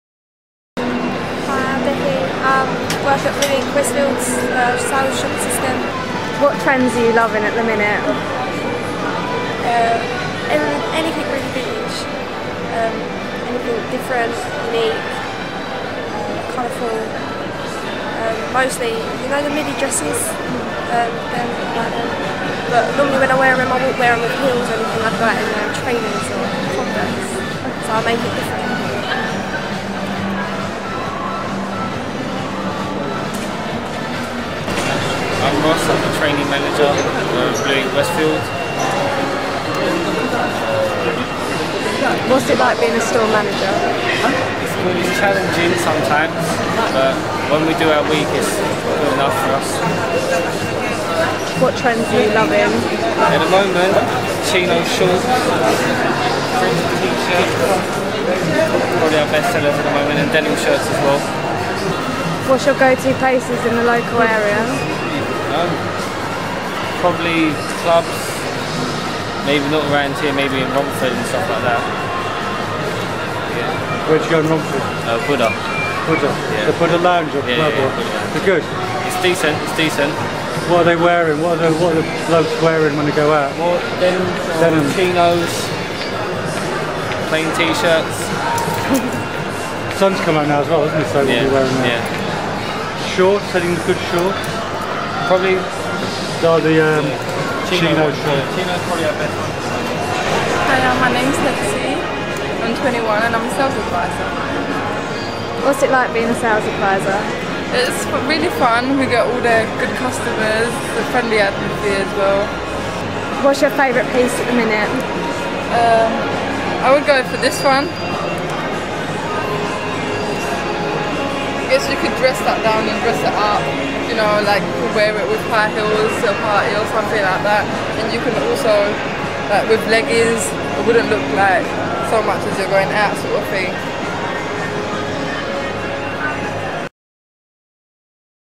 Blue Inc Chats to its Westfield Stratford Staff
We popped down to our store in Westfield Stratford to find out what it's like working for us here at Blue Inc and of course all things fashion!